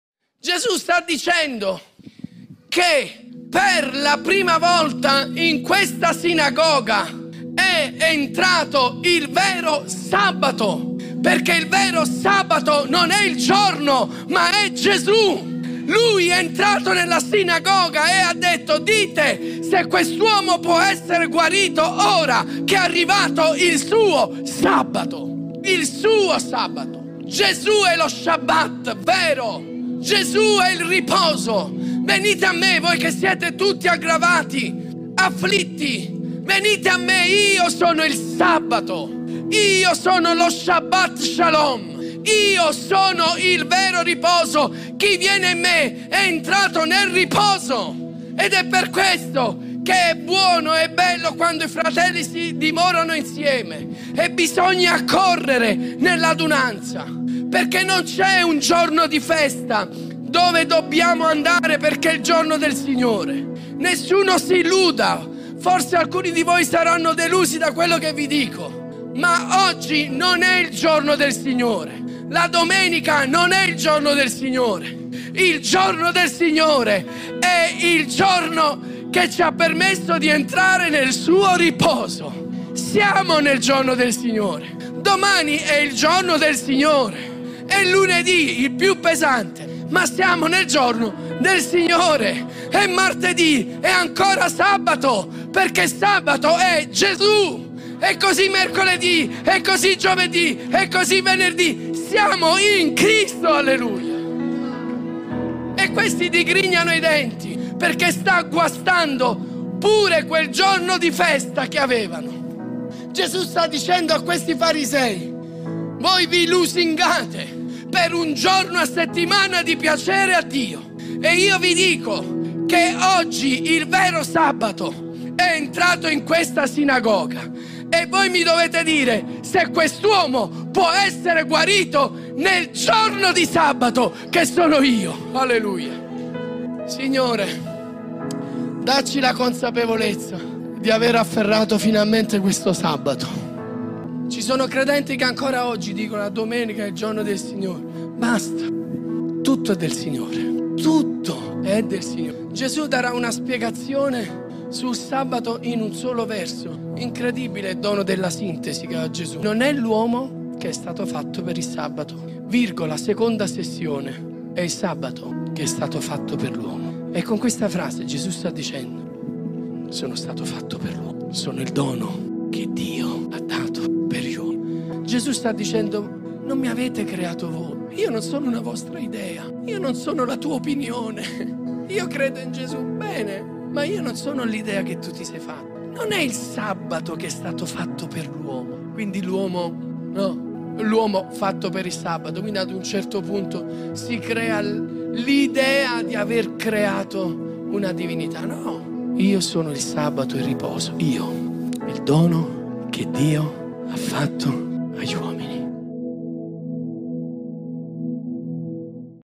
Il vero sabato
Il vero sabato - tratto dalla predicazione delle ore 17:00 del past. Gennaro Chiocca dell' 08 ottobre 2023.